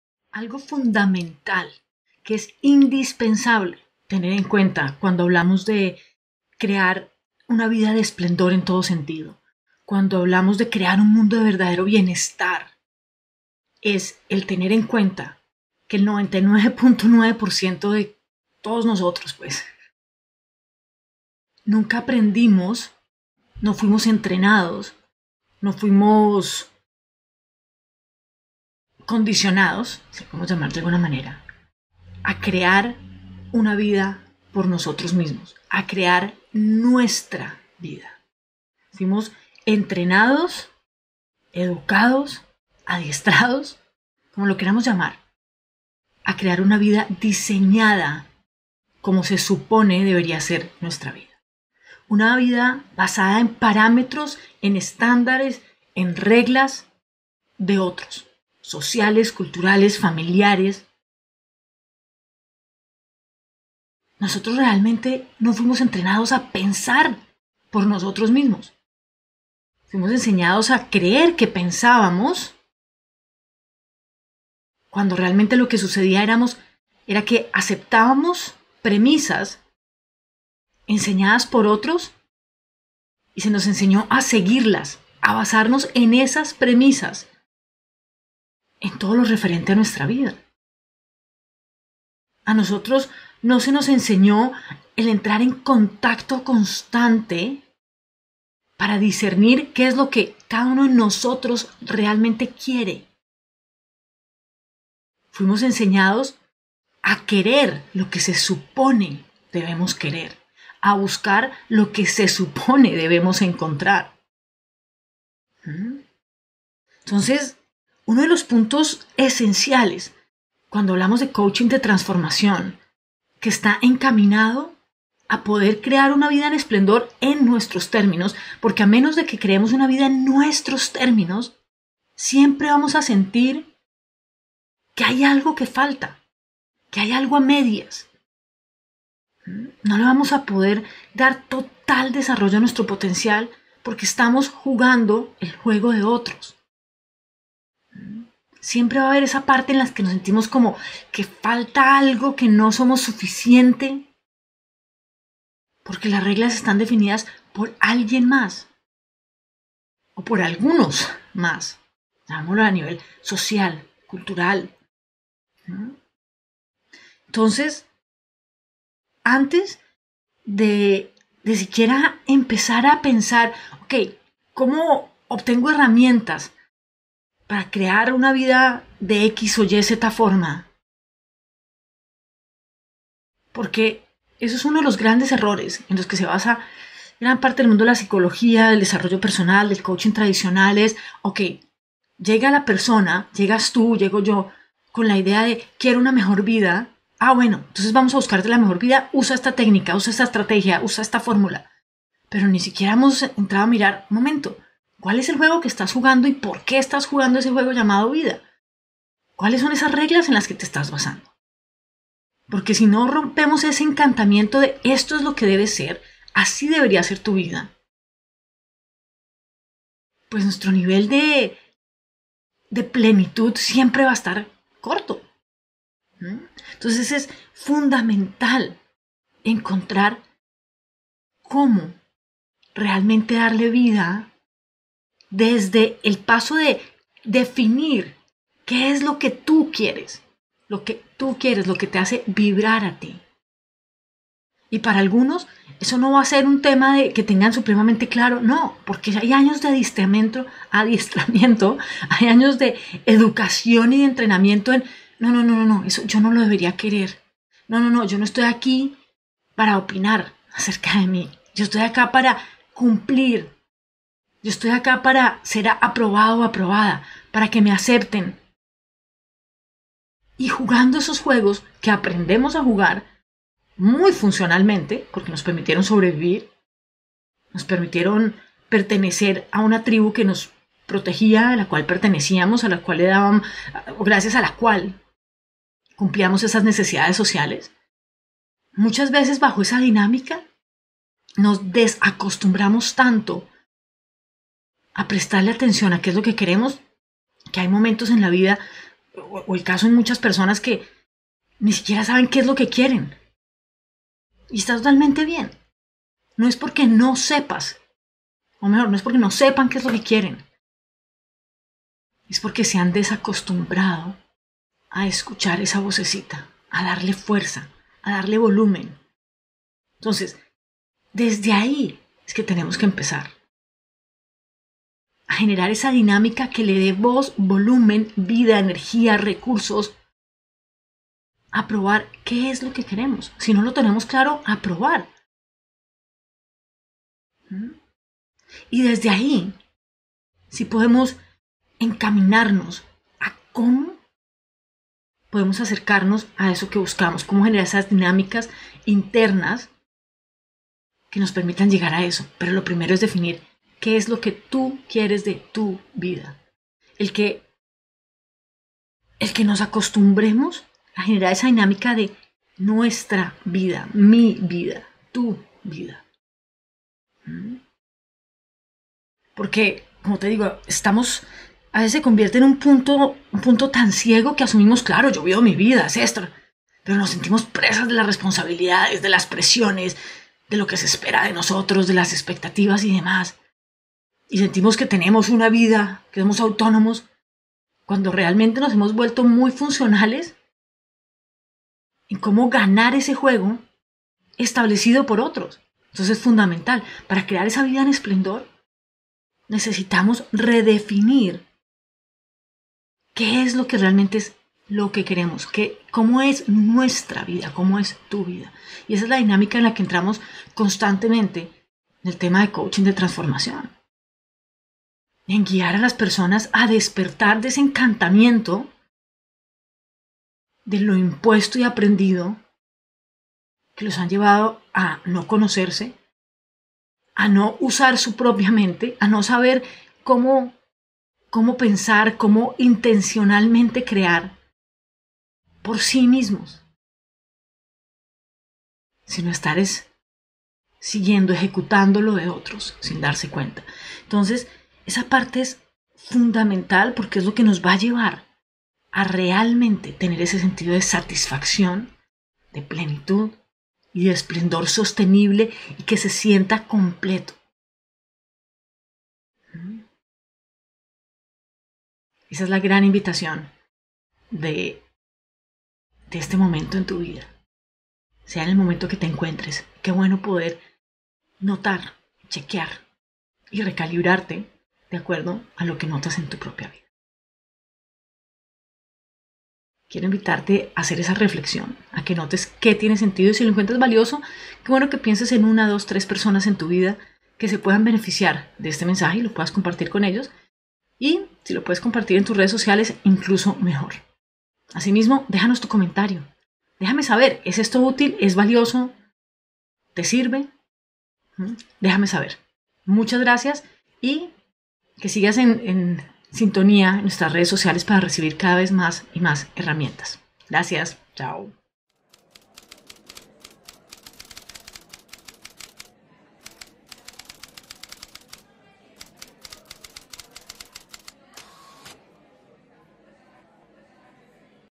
Aprende a vivir TU vida - Habilidades de Vida y Coaching ICG
Si quieres disfrutar una vida de esplendor, plenitud y bienestar en todo sentido, es indispensable que aprendas a vivir TU vida porque la realidad es que a la mayoría se nos entrenó para vivir una vida creada bajo los lineamientos, expectativas y estándares de otros.

Se nos enseñó a “obedecer” y a cumplir con reglas y por eso es tan común encontrarnos que a pesar de que hacemos todo lo que se supone debemos hacer y lograr, queda aún la sensación de que algo falta.

En este video comparto contigo de qué se trata este fenómeno y qué puedes hacer al respecto para crear la vida que quieres y para ayudar a otros a hacer lo mismo. Algo esencial a título personal y como coaches y guías en la vida de otros.